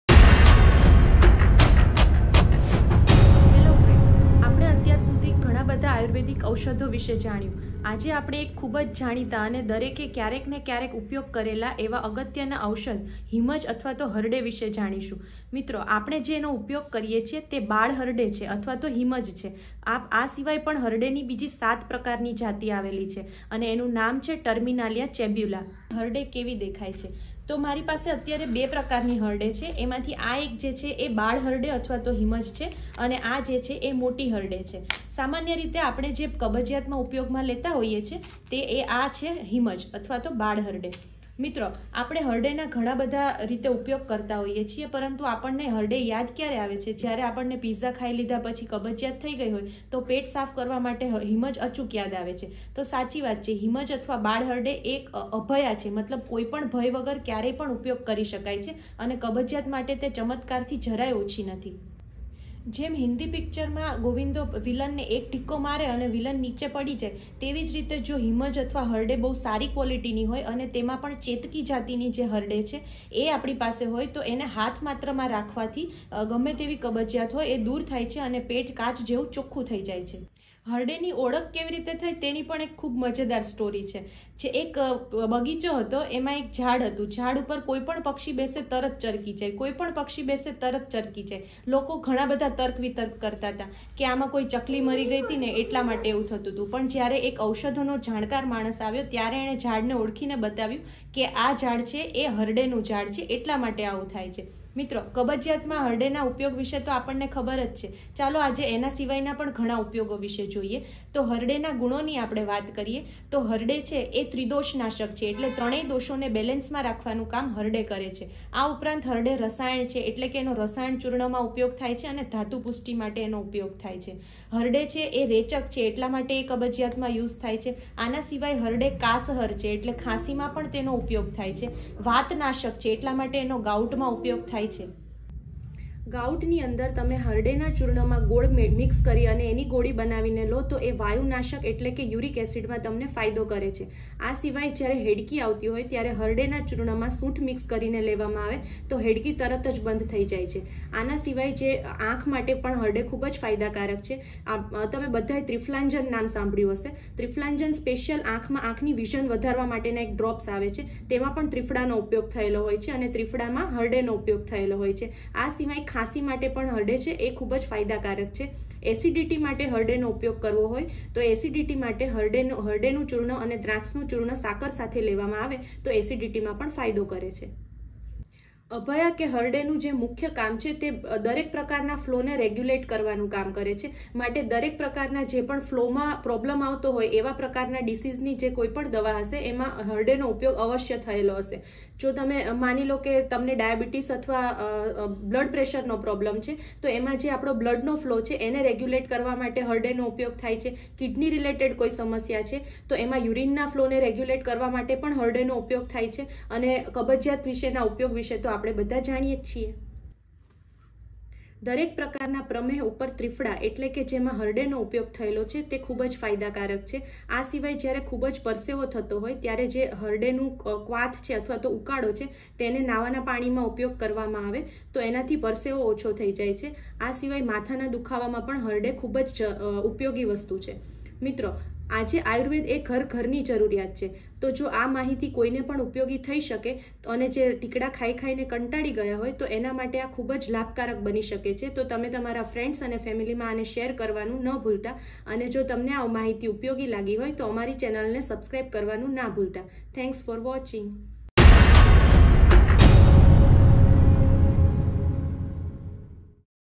Harade
હરડે
આમ તો હરડે ની સાત જાત છે
આપણે સામાન્ય રીતે બાળ હરડે ઉપાયોગ માં લઈએ છીએ અને એ પણ ખાલી પિત્ઝા ખાધા પછી કબજિયાત થઈ હોય તો એને દૂર કરવા
હરડે એ અભયા છે જેને આપણે કોઈ પણ ભય વગર લઈ શકીએ
હરડે કબજિયાત માટે ચમત્કાર થી ઓછી નથી
જેમ હિન્દી પીકચર માં ગોવિંદો એક ઢીકો મારી ને સમેવાળાને પાડી દયે એમ હરડે જો ખૂબ સારી હોય અને ચેટકી જાતિ ની હોય તો એને માત્ર હાથ માં પકડવાથીજ પેટ સાફ થઈ જાય છે આ હકીકત છે
એક બગીચામાં એક જાડ હતું એની ઉપર પક્ષી બેસે કે તરતજ ચરકી જતાં હતા અને લોકો જાત જાત ના તર્ક વિતર્ક લગાવતા હતા કેટલાક તો એવું કેતા કે અહી એક ચકલી મરી ગઈ ત્યાર થી આવુજ થાય છે પણ એક દિવસ એક વનસ્પતિ નો જાણકાર માણસ આયો અને એને લોકોને સમજાવ્યું કે આ હરડે ઉત્તમ પ્રકાર ની જાત નું ઝાડ છે
કબજિયાત કબજિયાત બોવ થયું હવે જોઈએ આપણે હરડે ના અસલી ગુણ
૧. ગાઉટ : હરડે ના ચૂર્ણ ને ગોળ માં ગોળી કરી આપવી
૨. હેડકી ઉપર: હરડે અને સૂંઠ નો પવદર કરી સૂંઠ સાથે ગરમ પાણી જોડે લેવાથી તરતજ હેડકી બંધ થાય છે
૩. એસિડિટી: હરડે ૧ ભાગ, દ્રાક્ષ ૧ ભાગ, સાકર ૨ ભાગ, અને એની ગોળી કરી લઈ સવાર સાંજ એક એક ગોળી ખવડાવવું
૪. આંખ ના નંબર ઉતારવા: ત્રિફલા નો કાઢો કરી એનાથી આંખ ધોવી
૫. ખાંસી: હરડે તથા બહેડા નું ચૂર્ણ મધ માં આપવું
૬. દરેક પ્રકાર ના પ્રમેહ માટે: ત્રિફલા હવે જો તમને ત્રિફલા એ ના ખબર હોય તો હું એક વિડીયો માં ડિટેલ માં સમજાવીસ પણ અત્યારે એટલું જાની લઈએ કે ત્રિફલા દરેક પ્રકાર ના પ્રમેહ માં ઉપાયોગ માં આવે છે.
૭. વધુ પરસેવો આવતો હોય તો હરડે ને પાણી માં વાટી અને શરીરે ચોપડાવી અને પછી નાવું એટલે પરસેવો વળવાનું ઓછું થઈ જશે
૮. દરેક પ્રકાર ના માથા ના દૂખાવા માં: હરડે, કરિયાતું, હળદર, અને ગાળો નો ઉકાળો કરી ટેમ છઠા ભાગ નો ગોળ નાખી પીવો એટલે આંખ, કાન અને માથાનો દૂખાવો માટે છે.
આયુર્વેદ એ ઘરે ઘર ની જરૂર છે એટલે જો તમારી આજુબાજુ ના લોકો ને આમાંની કોઈ પણ સમસ્યા હોય અને ટિકડા ખાઈ ખાઈ ને કંટાળી ગ્યા હોય તો એમને એક વખત શેર કરજો બની શકે કે એમને મદદ રૂપ થઈ જાય
અને જો તમને હરડે વિષે ની માહિતી ઉપયોગી લાઇગી હોય તો મારી ચેનલ ને સબસક્રાઇબ કરજો
અને હવે બીજા વિડીયો માં આપણે મલસું અને બહેડા વિષે જનસુ